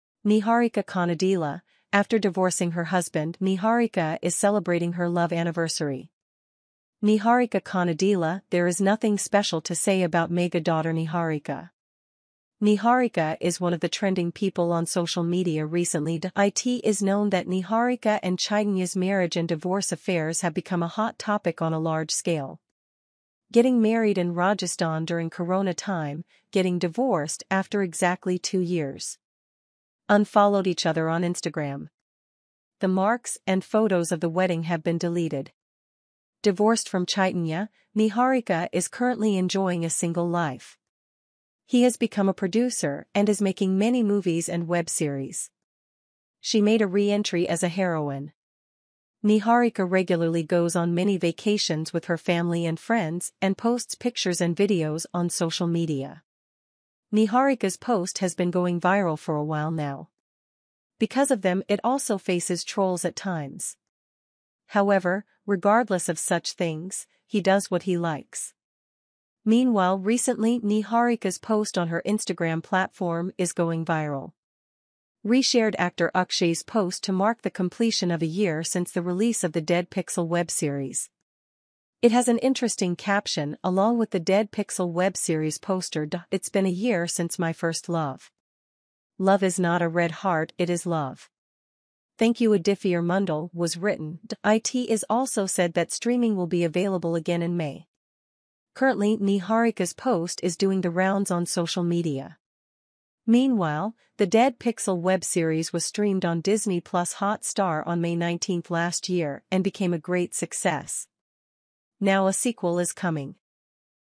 Niharika Konidela is celebrating her love anniversary after divorcing her husband.
Niharika Konidela, a mega daughter of Chaitanya Konidela, celebrated her love anniversary on social media after divorcing her husband. She now enjoys a single life, producing movies and web series, and regularly goes on vacations. Akshay's post commemorated the Dead Pixel web series' release, which was successful and is set to return in May.